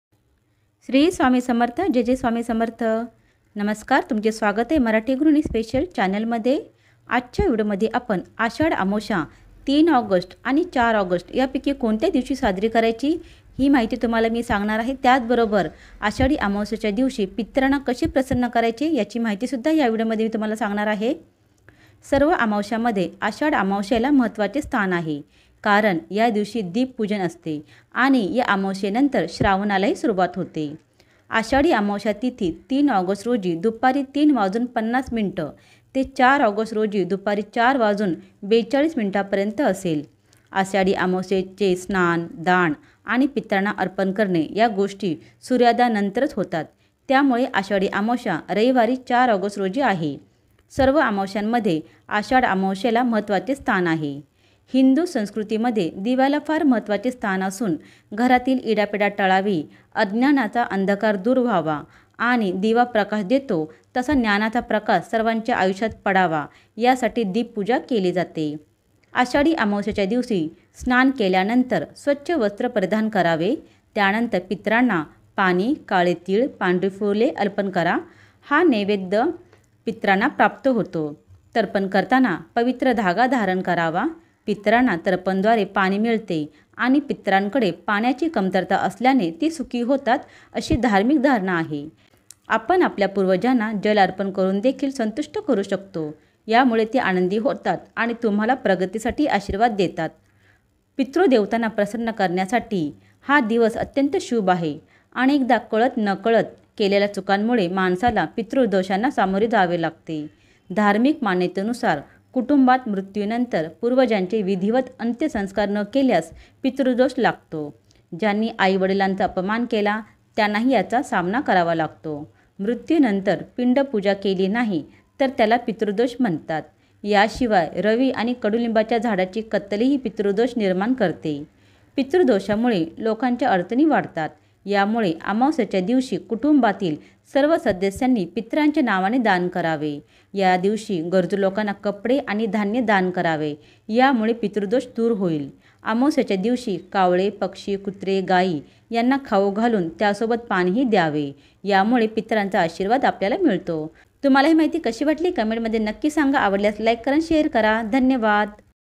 आषाढ अमावस्या २०२४ कधी आहे | आषाढ अमावास्येला पितृदोष कसा दूर करावा / Ashadh Amavasya in Marathi
यंदा आषाढ अमावस्या कधी  | Aashad Amavasya (Dip Amavasya) 2024 Sampurn Mahiti

ashadh amavasya kadhi aahe | आषाढ अमावास्येला पितरांना प्रसन्न कसे करावे